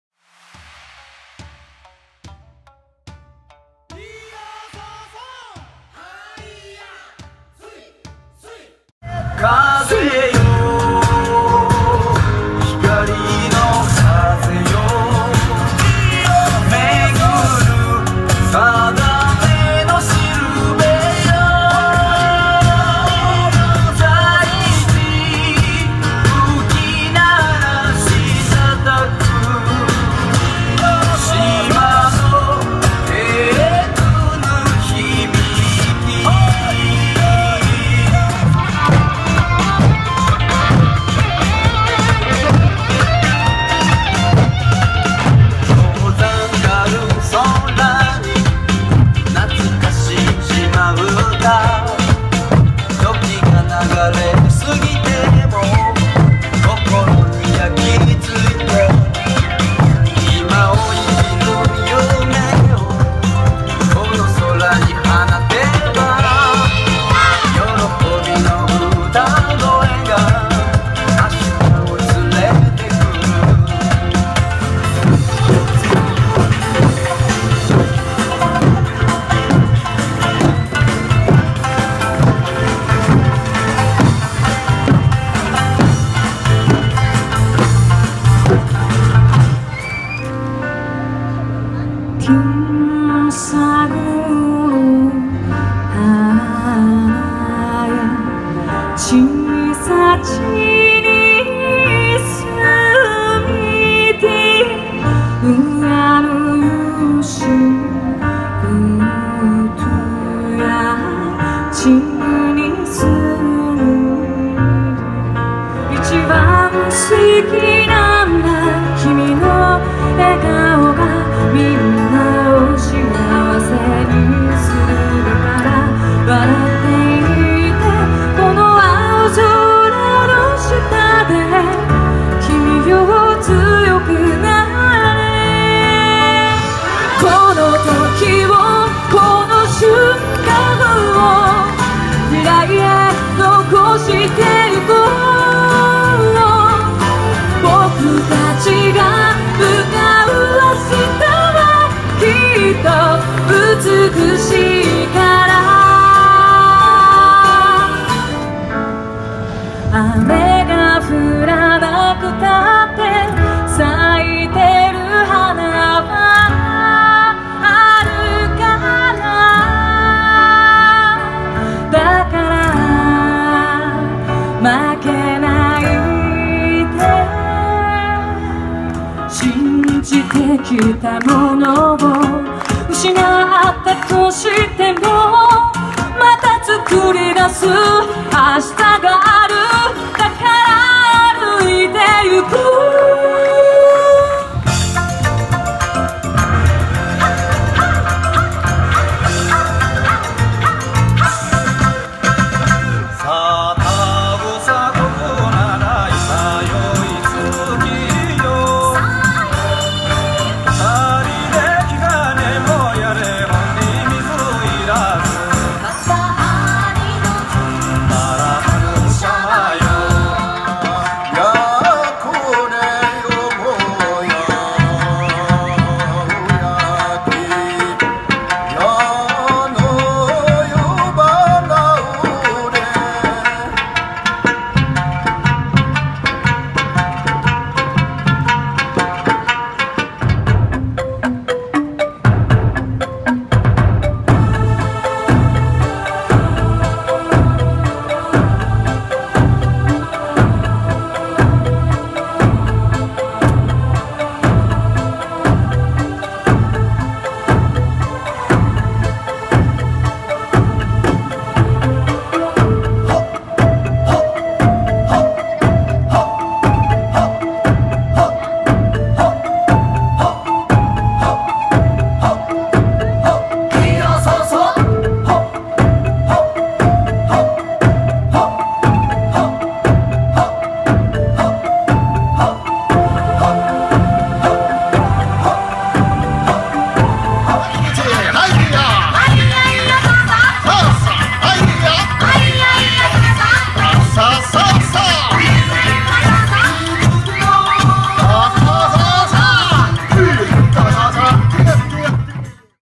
あったかいさぁー おいしいさぁー 旬をまるごと沖縄フェア2015
3月15日（日）、東京・有楽町駅前広場で開催の「あったかいさぁー おいしいさぁー 旬をまるごと沖縄フェア」は、沖縄から直送の野菜や果物を販売。これから観光の旬を迎える沖縄の魅力がたっぷり紹介された。ステージでは、沖縄のエイサーや演舞のほか、音楽ライブも行われた。主演：炎舞太鼓、しおり、琉球舞踊

撮影・編集　iphone5S